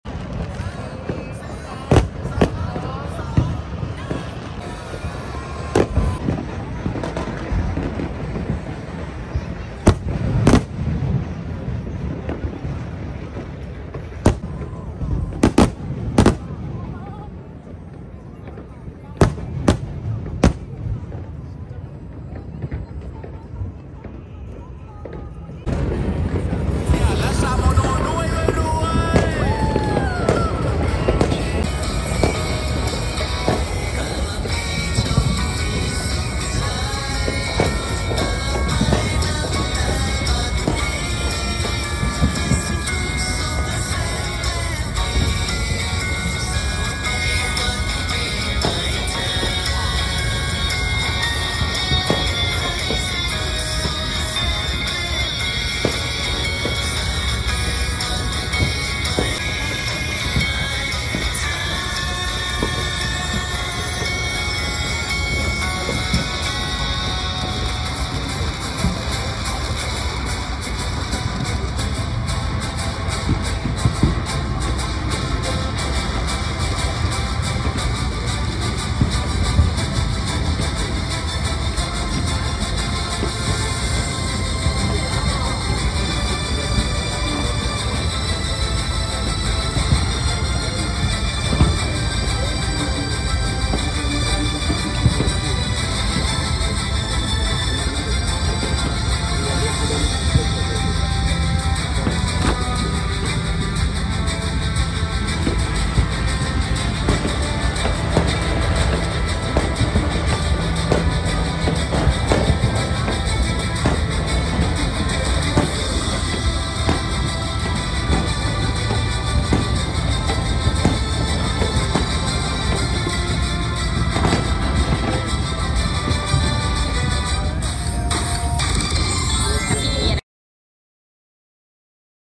Kid watching Happy new year2023🎈🎉 non stop fireworks at pattaya beachThailand🎊
Non stop fireworks ❤️Happy New year🎉🎉🎊Pattaya beach